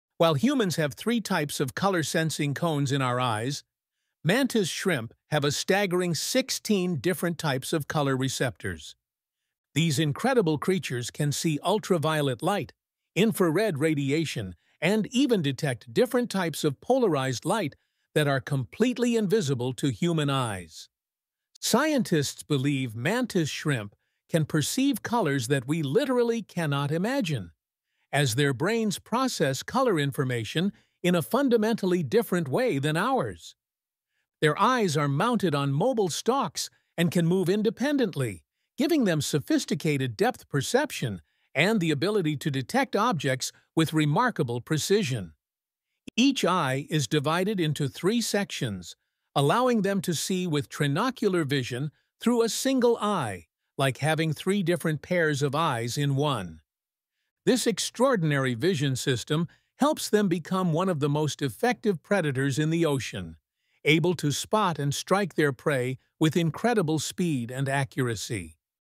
Unlocking the Secrets of the Mantis Shrimp's Incredible Vision 🌈🔍
Dive into the extraordinary world of the mantis shrimp and discover how its mind-blowing vision sets it apart from humans! 🌊✨ With 16 types of color receptors, these underwater marvels can see colors and patterns beyond our wildest imagination. From detecting polarized light to their unique trinocular vision, find out why mantis shrimp are among the ocean's ultimate predators. Get ready for a visual treat filled with stunning macro footage and fascinating insights!

🔹 What do you think about mantis shrimp's crazy vision?